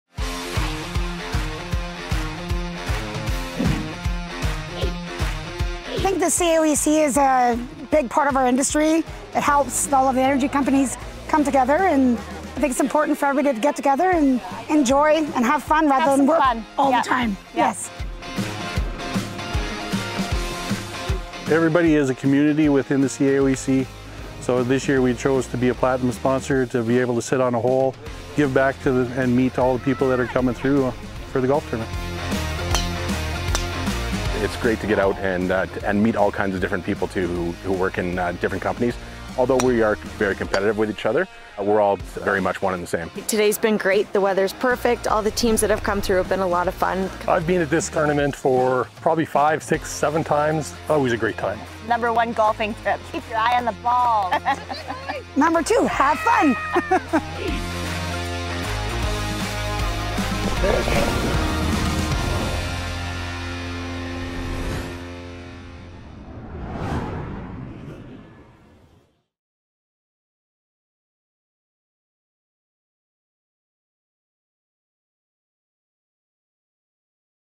CAOEC Golf Tournament Highlights
The annual CAOEC Golf Tournament is a great opportunity for CAOEC members to enjoy a fun-filled day of golf, food, and great networking opportunities.

To find out more about the perks of becoming a member, visit the CAOEC